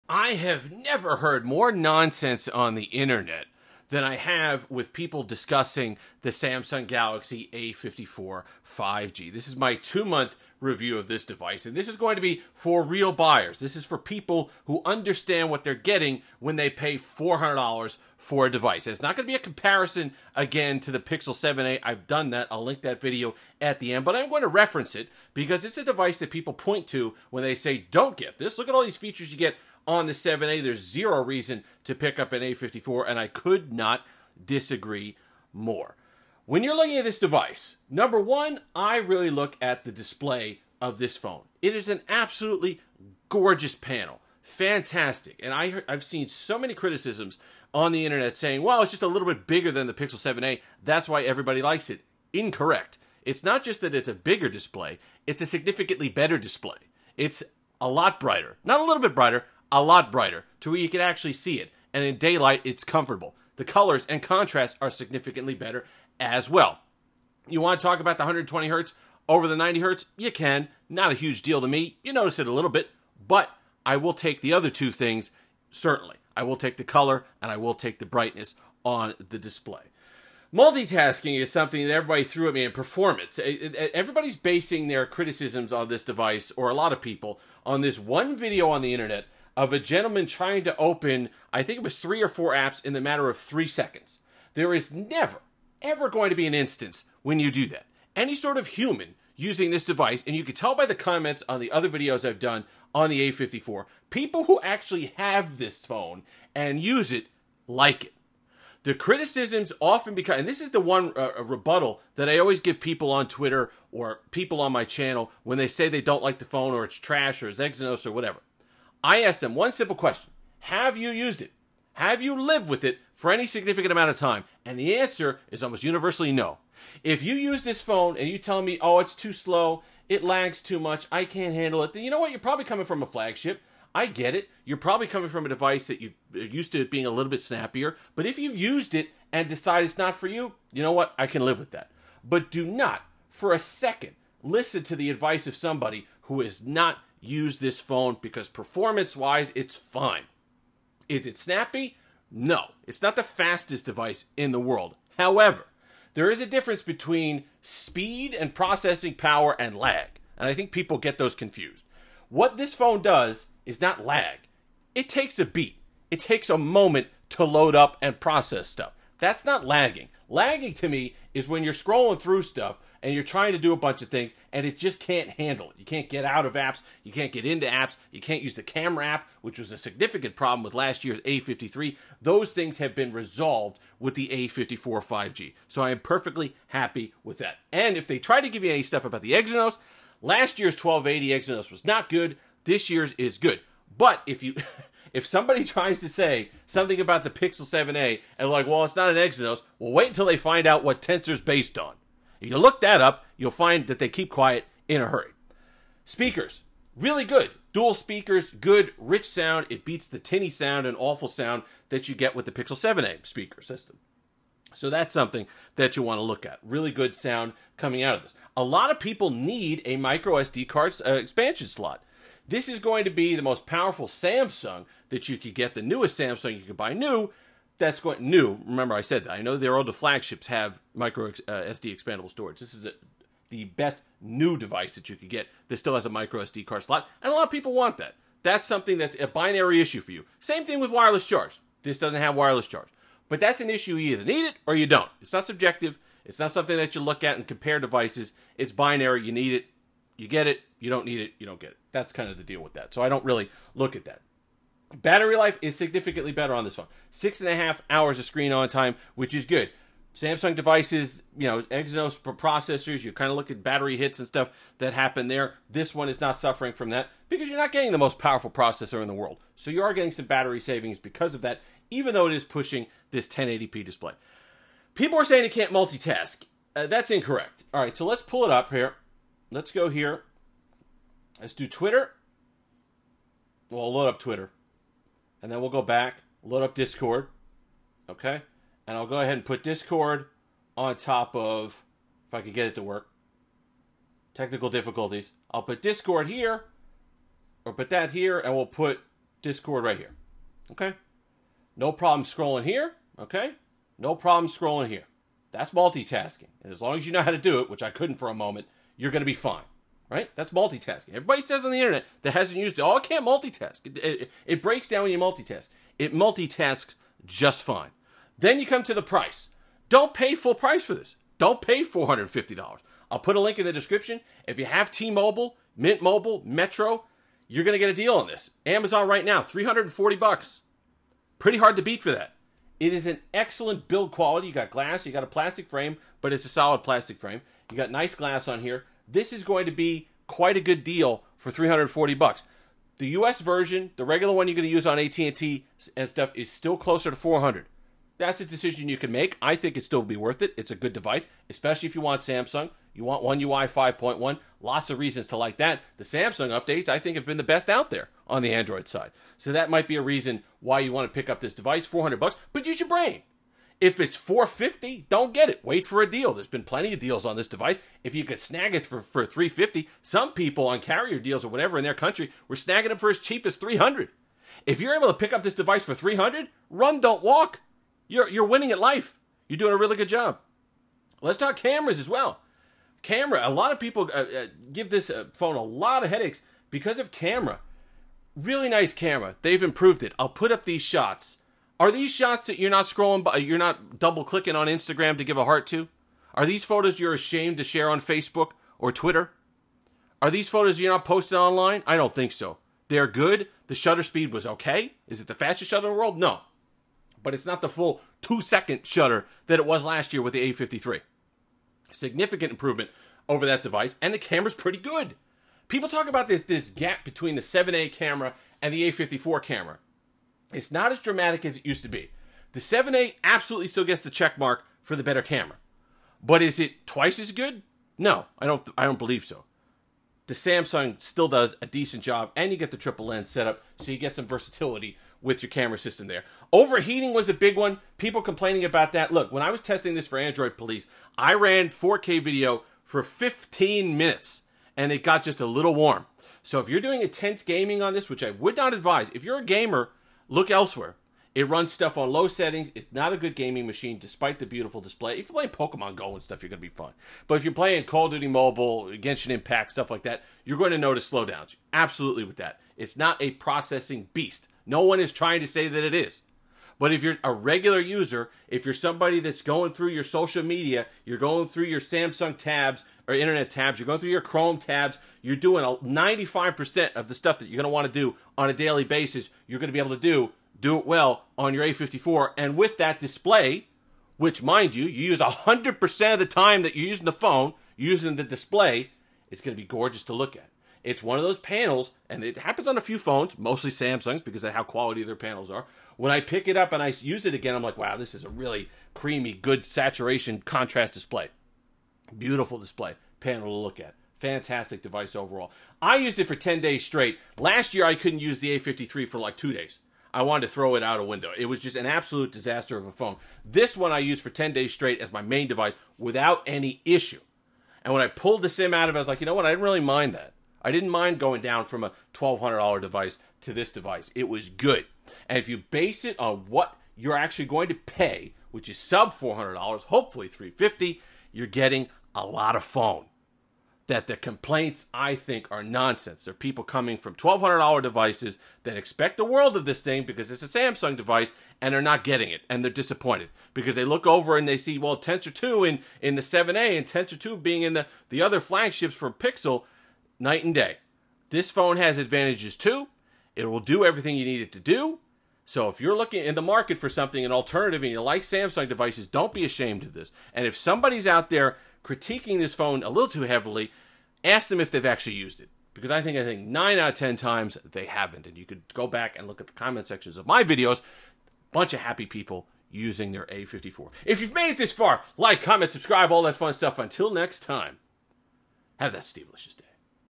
Samsung Galaxy A54 5G after Two Months!
My thoughts on the Samsung Galaxy A54 5G after two months of use. I still really like it. If you are honest with yourself about what the phone provides, you will be really happy, and it absolutely has a lot of reasons why people would want to purchase it. Lots of people who are hating on it, never even used it, so make sure to see through that!